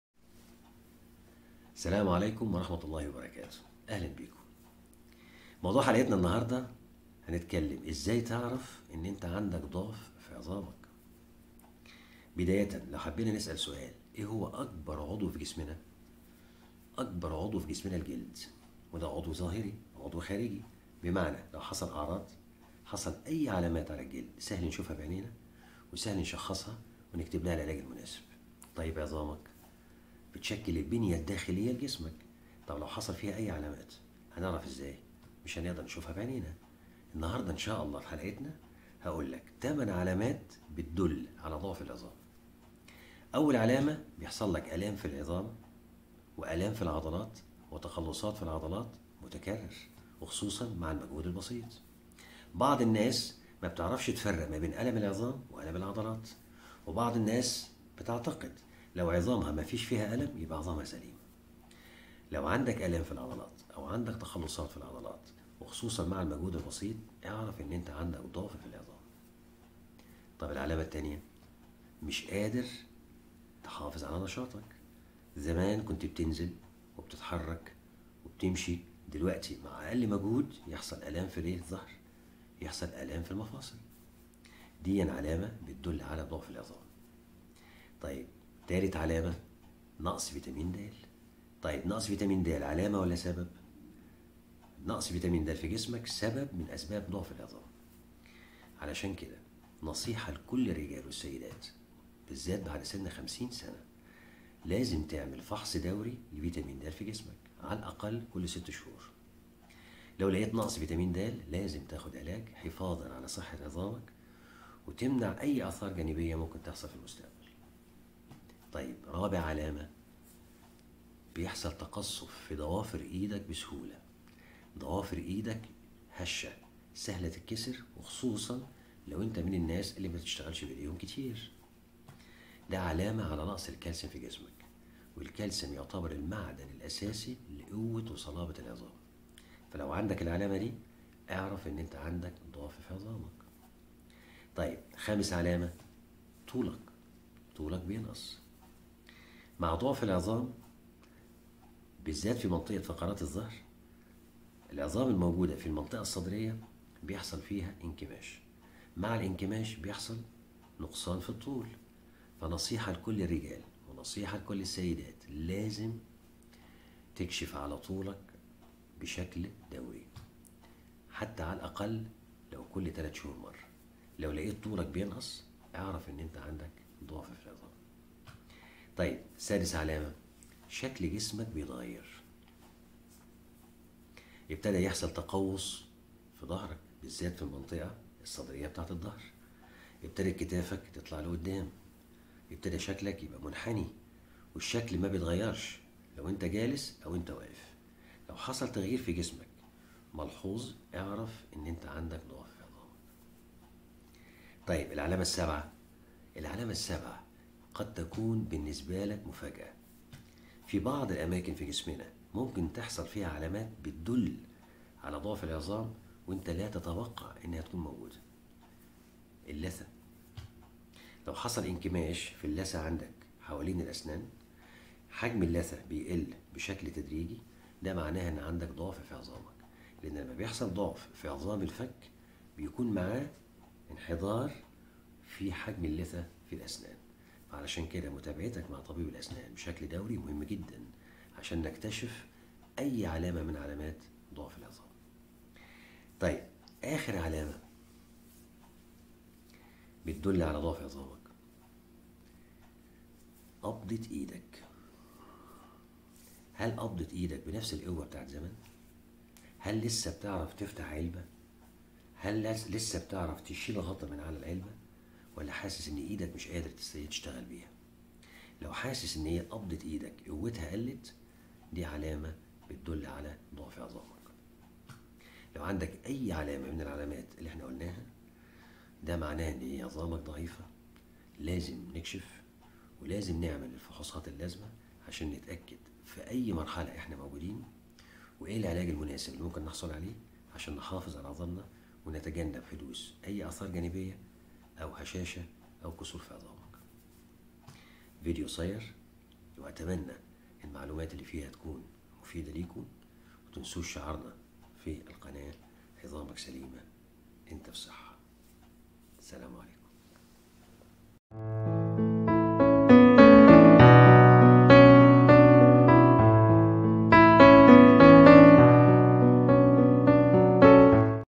٨ علامات تدل علي ضعف عظامك د حاتم أمين أستشاري جراحة العظام و المفاصل
العظام لتشكل البنية الداخلية لجسمك عند حدوث أي علامات لا يمكن معرفتها بسهولة
العلامات الأولية التي تدل علي ضعف عظامك
ظهور العلامات تساعدك علي التشخيص المبكر لضعف العظام
المصادر
8 Common Signs and Symptoms - The 8 Signs of Osteoporosis